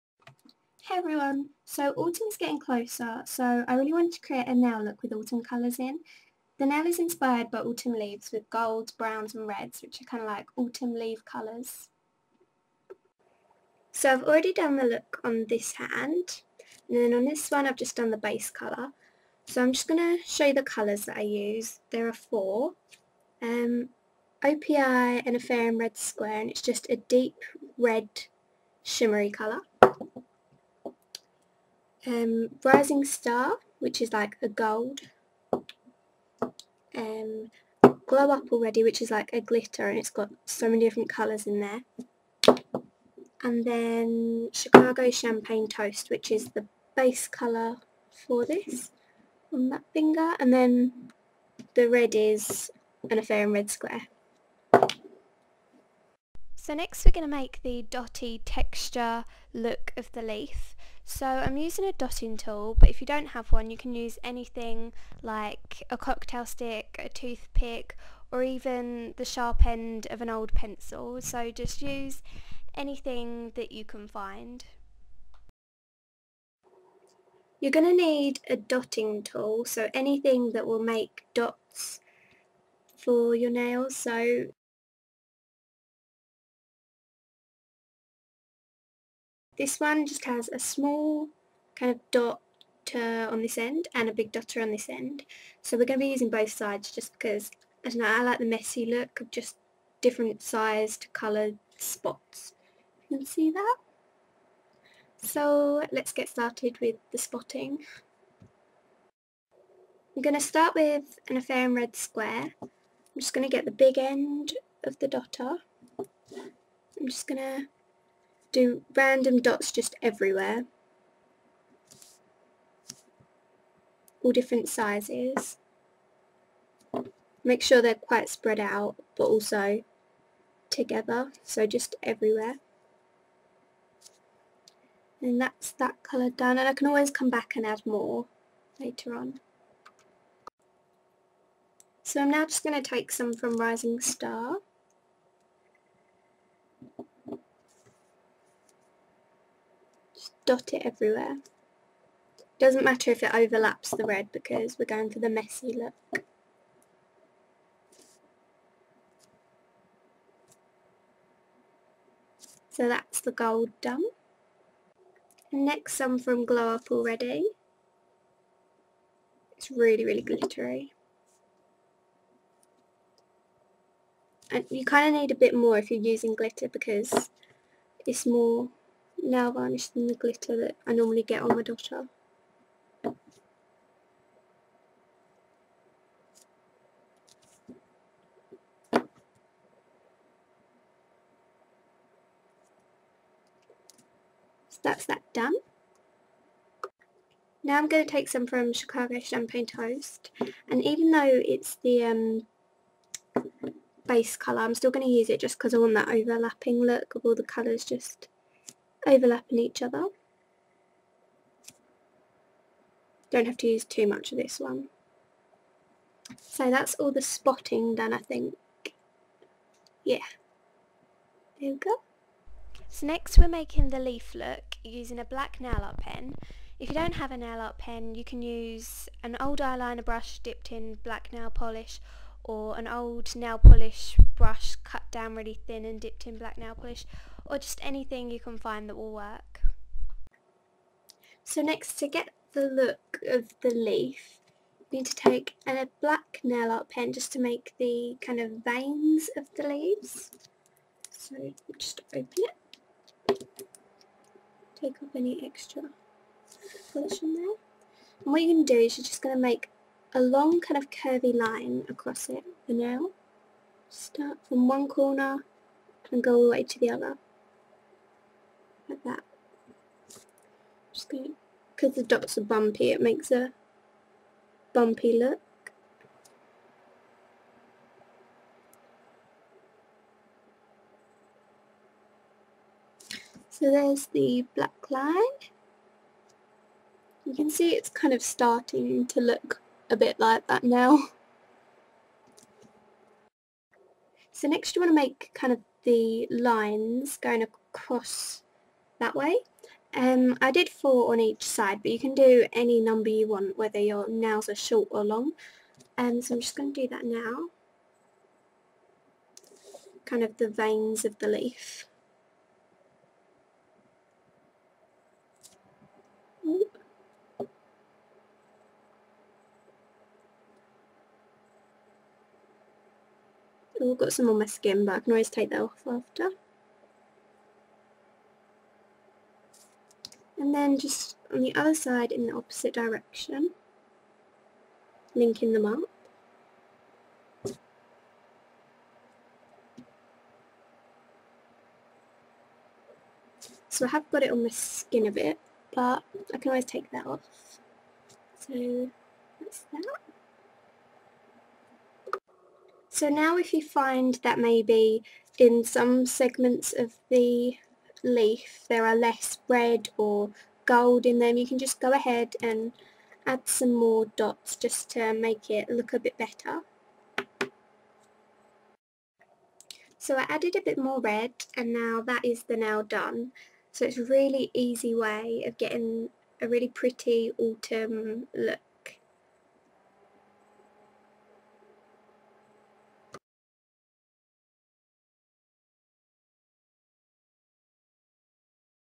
Autumn Fall Inspired Nail Tutorial
A pretty simple nail art inspired by autumn and golden leaves!

Colours used-
O.P.I An affair in red square
O.P.I Chicago champagne toast
O.P.I Rising star
O.P.I Glow up already